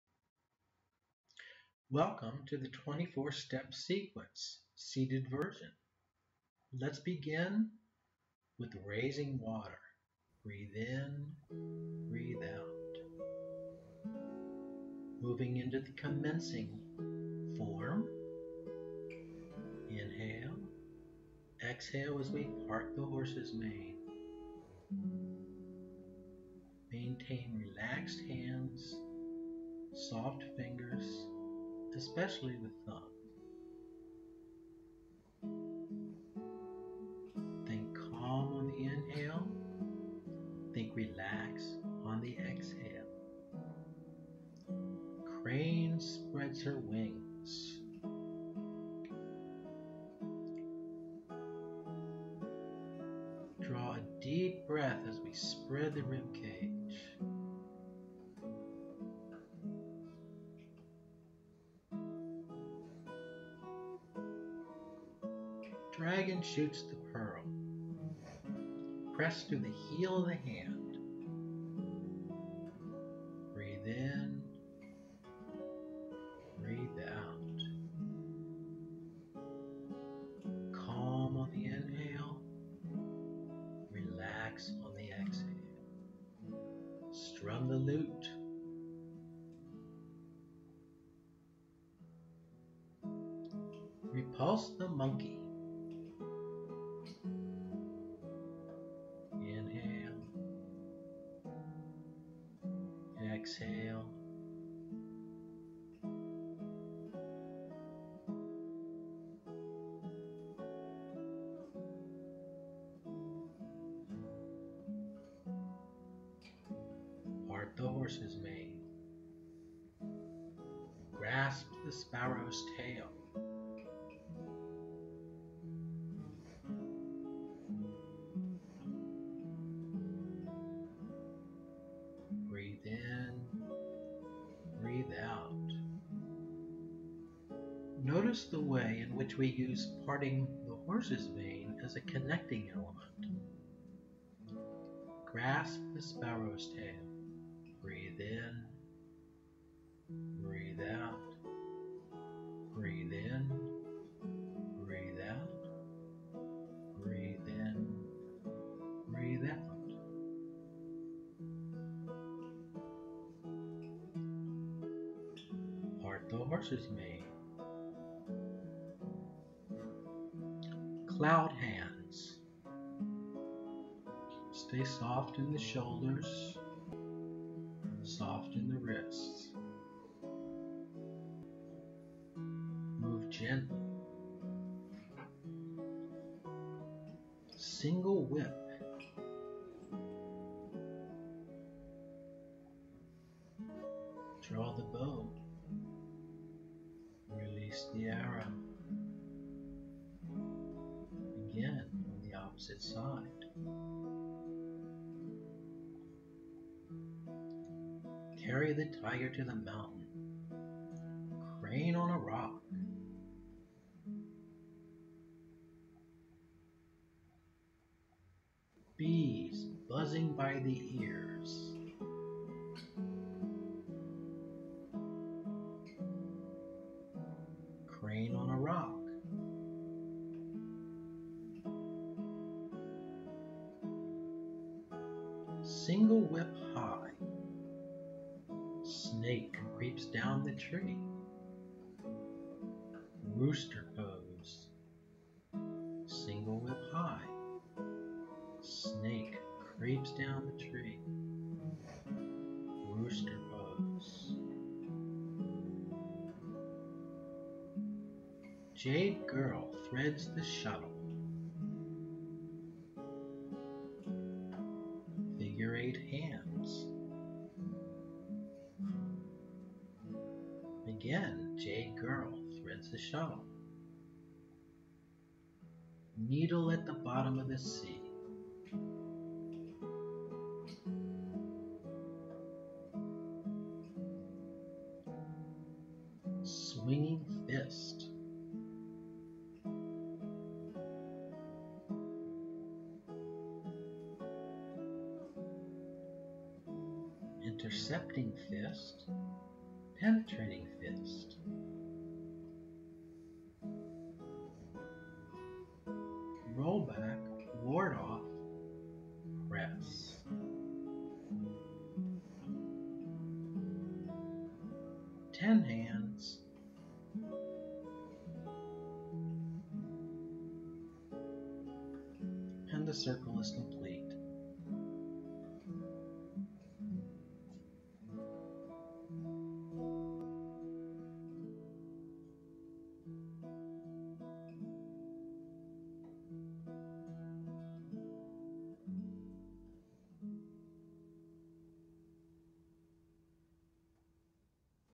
Seated Tai Chi Chuan #3 - Twenty Four Movements
This is a seated version of the twenty four element (step) short form.